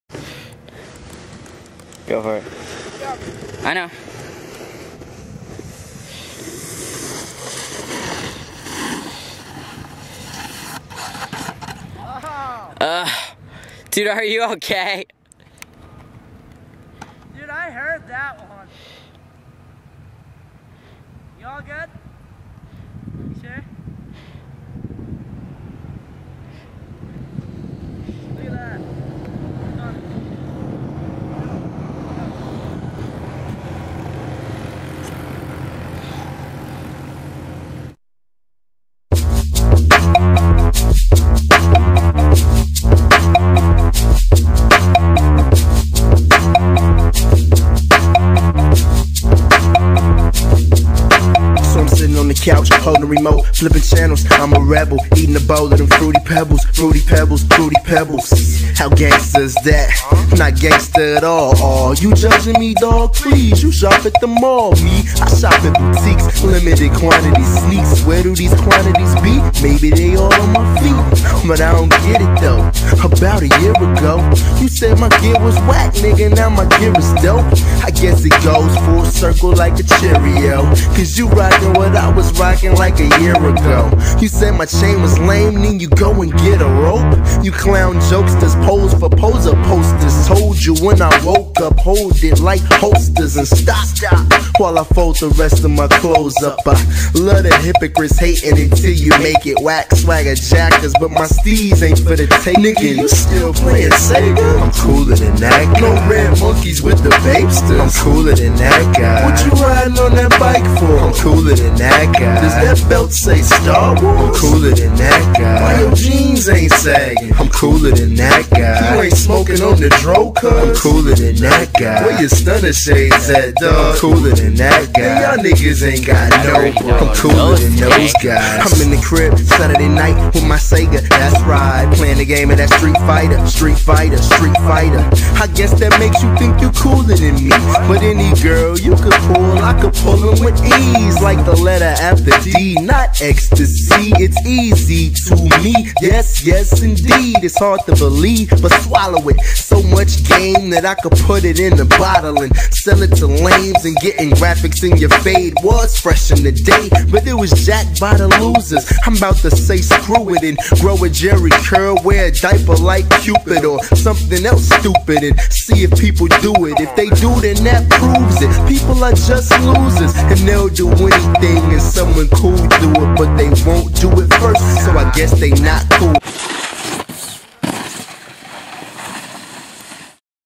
pre season
shreddin blown snow before the mountain opens